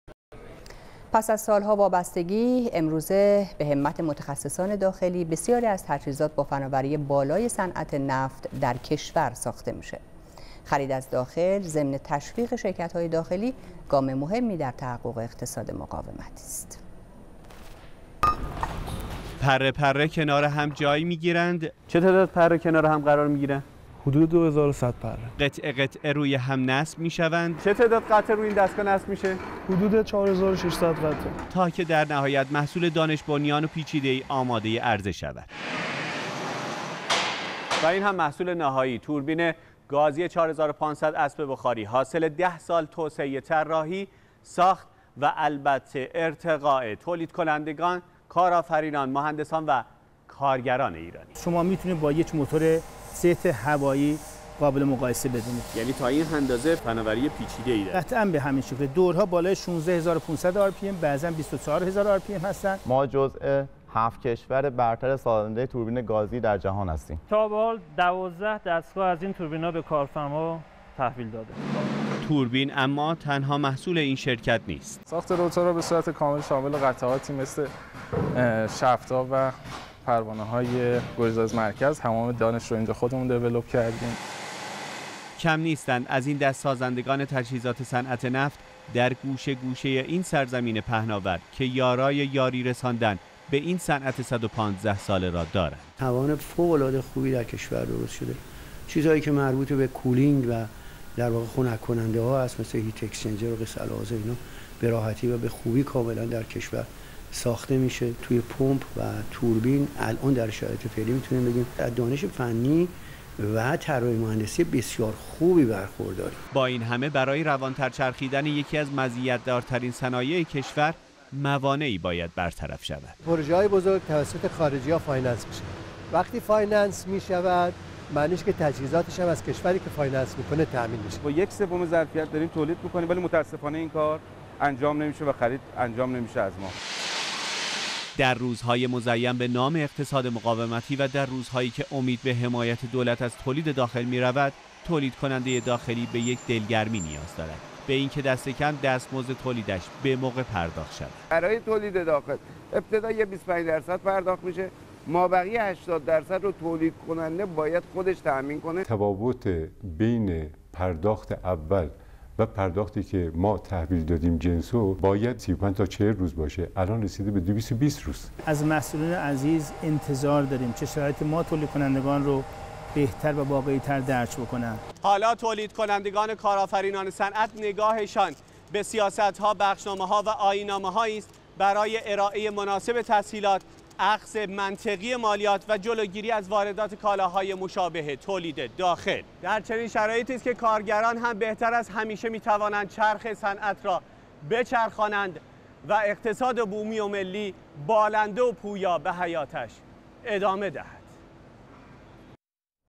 تجهيزات نفتي بومي - عليرضا باي
تجهیزات نفتی بومی - علیرضا بای

پس از سال ها وابستگی، امروزه به همت متخصصان داخلی، بسیاری از تجهیزات با فناوری بالای صنعت نفت، درکشور ساخته می شود. خرید از داخل، ضمن تشویق شرکت های داخلی، گام مهمی در تحقق اقتصاد مقاومتی است.